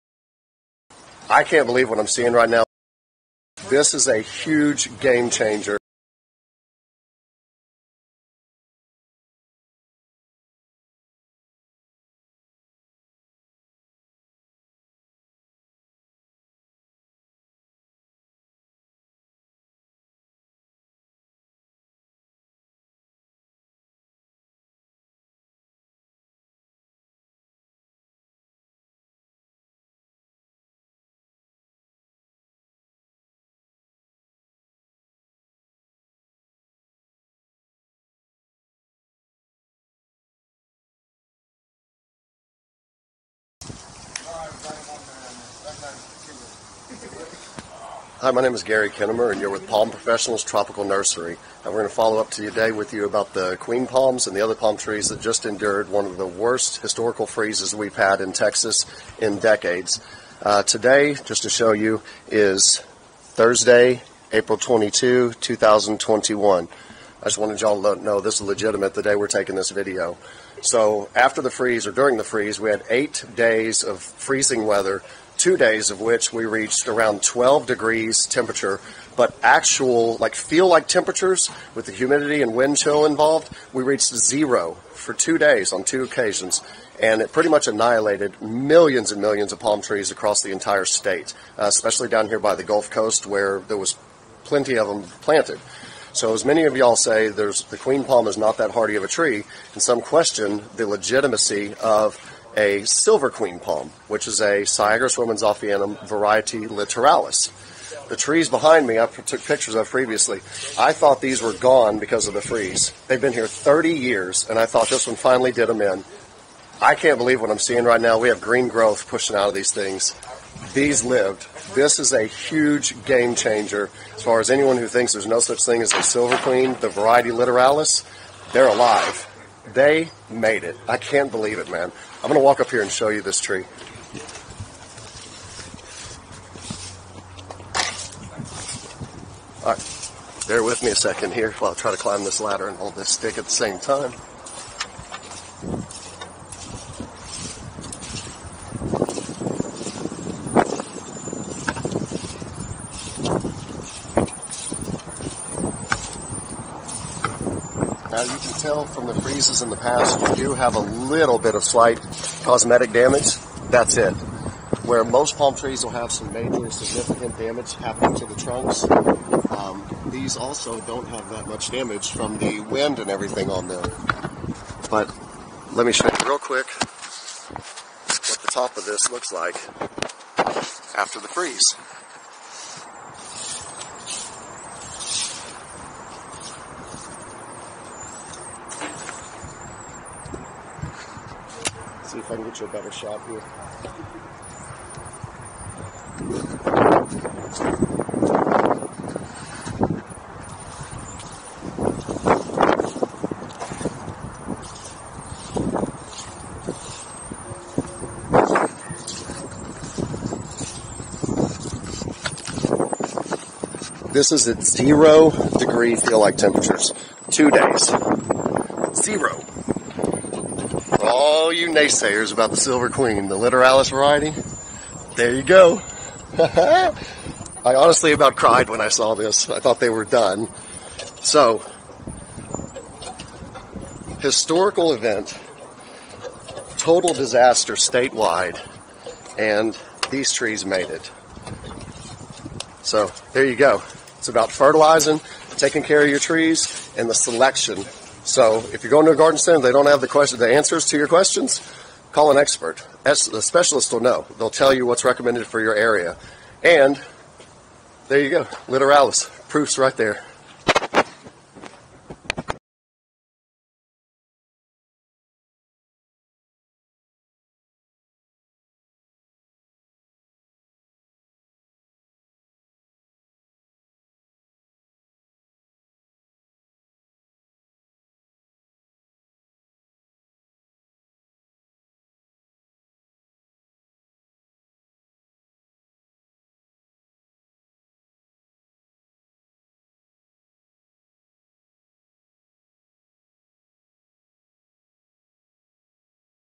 Palm Tree Record! Silver Queen Palm Record Breaking Freeze Resistance The Most Cold Hardy Queen Palm
HUGE Discovery Today In The Palm Tree Industry!!!

Watch Till The End For The Full Explanation

Historic Freezes Created A Disaster During February 2021 That Killed Millions Of Palm Trees...Or Did It???

Even Many Of The Hardiest Palms That Have Been Here For Decades Died So It’s No Surprise That The Moderately Delicate Ones Were The First To Go….
So We Thought…Until Now.

A Silver Queen Palm (Syagrus Romanzoffiana var. Litoralis) Survived 0 Degree Feel Like Temperatures For 2 Days And Is Pushing Very Strong New Growth After A Solid Week Of Low Teen Temperatures And More Importantly The 2 Days Of Negative Below Zero Feel Like Temperatures…
This Has Never Been Documented Before In Any Queen Palm Variety!!!

DOCUMENTED:
Palm Specialist In The Video:
Gary Kennemer

Location:
Sugar Land, Texas

Fertilizer Used:
Perfect Palm Fertilizer

Palm:
Silver Queen Palm (Syagrus Romanzoffiana var. Litoralis)

Documentary By:
 Palm Professionals Tropical Nursery
20113 Southwest Freeway
Sugar Land, Texas 77479